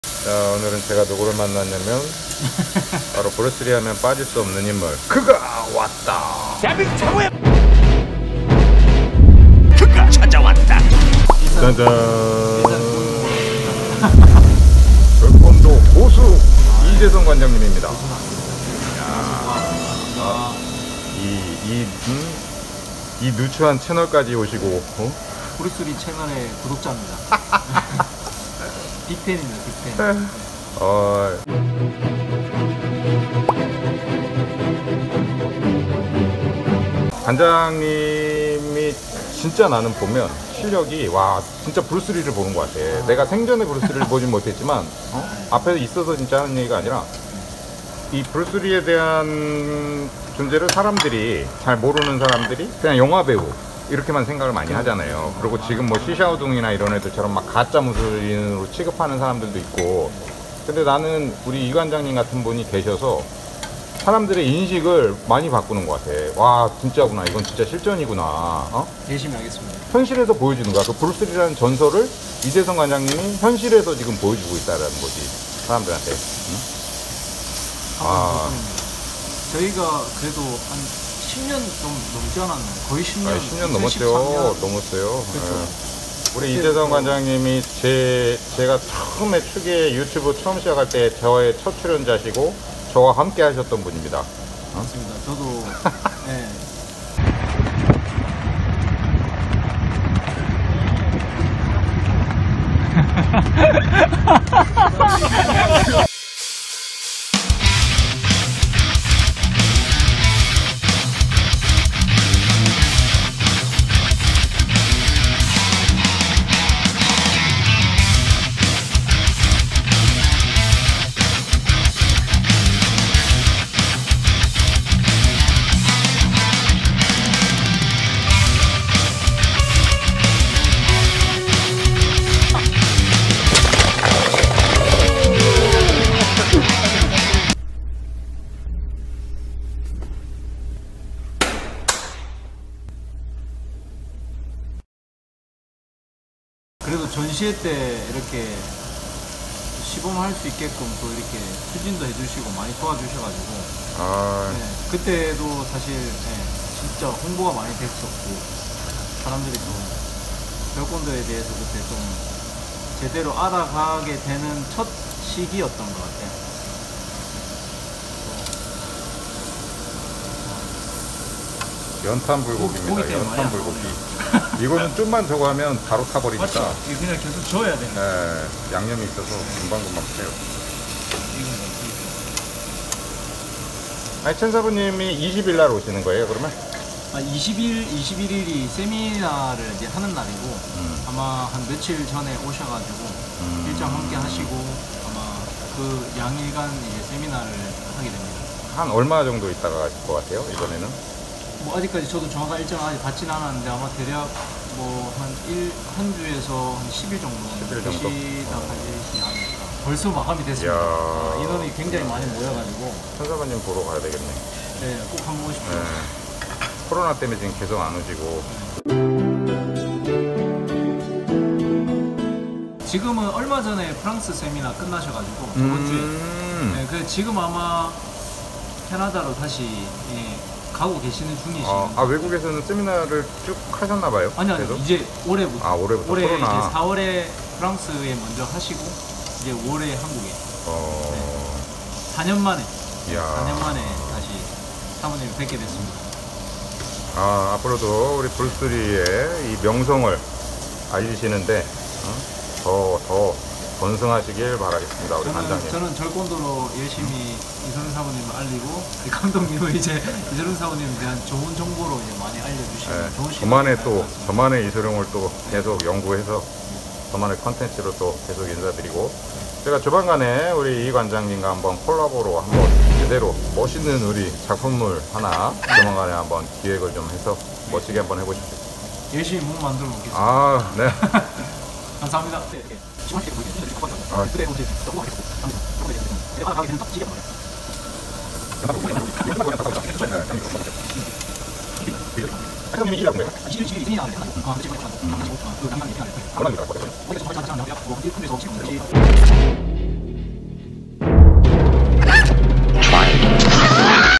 🔥 절권도 쌉고수 이재성 관장님 연탄불고기 ㅋㅋ
일산 급만남 돌발촬영! 절권도 이재성 관장님